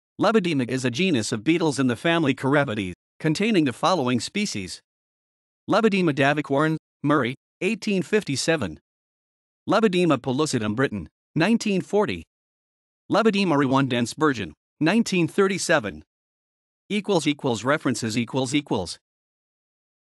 Lebidema
Lebidema is a genus of beetles in the family Carabidae, containing the following species:
Lebidema davicorne (Murray, 1857)
Lebidema pellucidum Britton, 1940
Lebidema ruandense Burgeon, 1937

== References ==